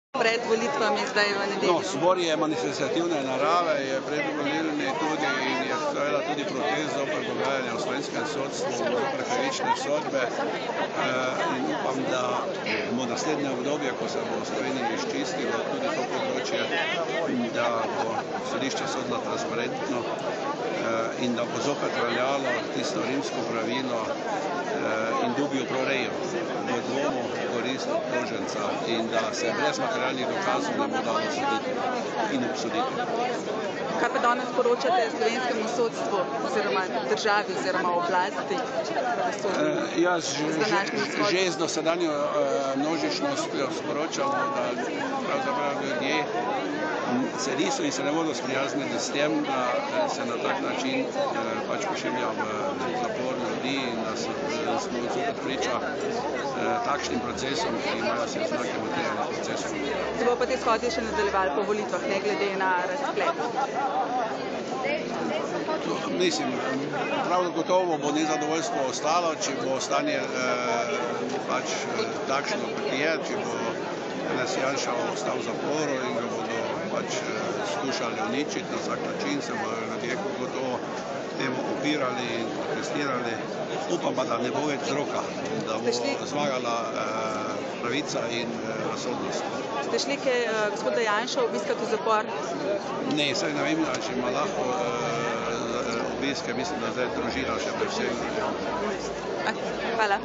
SLOVENSKE NOVICE: Shod za Janševo svobodo - izjava Jožefa Jerovška
slovenskenovice.si